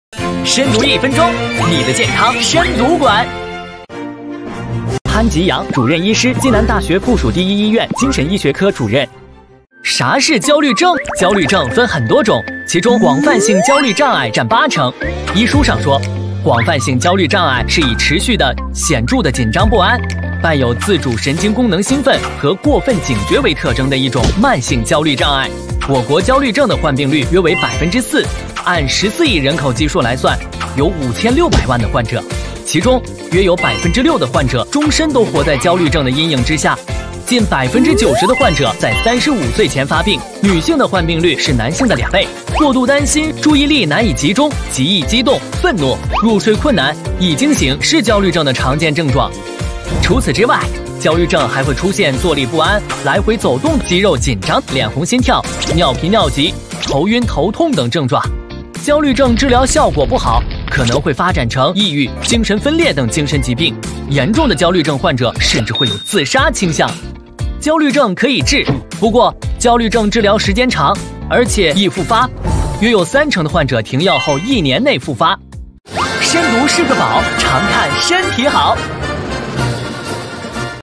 深读小视频-焦虑症（01）什么是焦虑症？
什么是焦虑症？焦虑和焦虑症是一回事吗？恐惧和担忧是焦虑吗？需要治疗吗？焦虑情绪会发展成焦虑症？多少岁前易患焦虑症？焦虑症患病率女性比男性高2倍？焦虑症会出现注意力难集中，易激动、愤怒的症状？入睡困难、易惊醒，尿频尿急、头晕头痛也是焦虑症的常见表现？焦虑症可能会发展成抑郁、精神分裂？甚至会有自杀倾向？